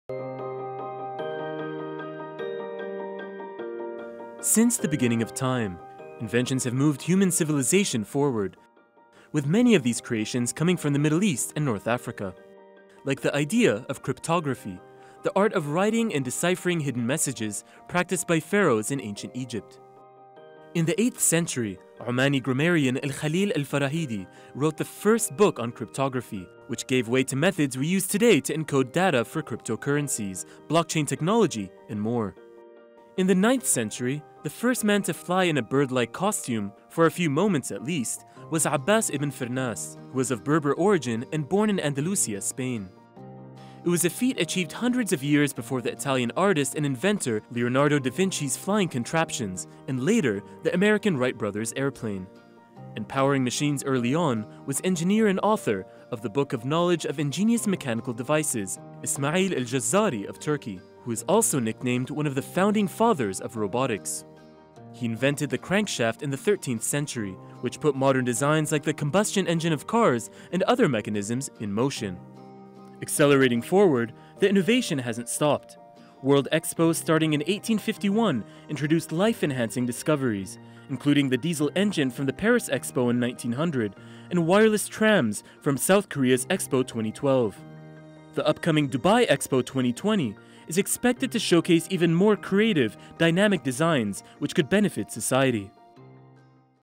What are some Middle East inventions that helped shape the world?
Since the beginning of humankind, inventions have helped move civilizations forward, with many creations coming from Middle East and North Africa.…